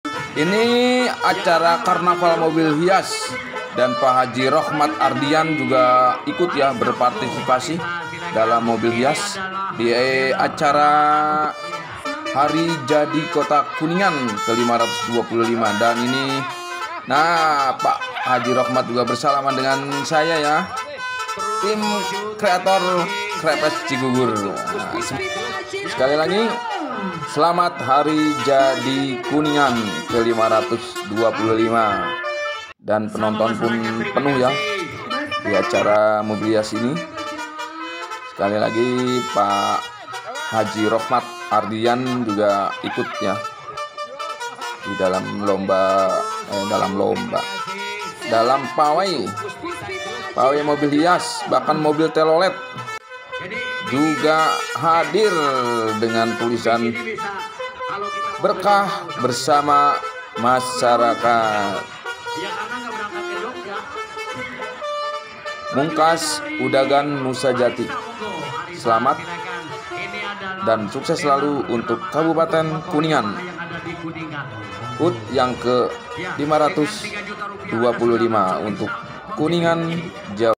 BUS TELOLET H ROKHMAT ARDIYAN MM ADA DI KUNINGAN KE 525
Kuningan ke 524 ada bus telolet H Rokhmat Ardiyan mm di Karnaval mobil hias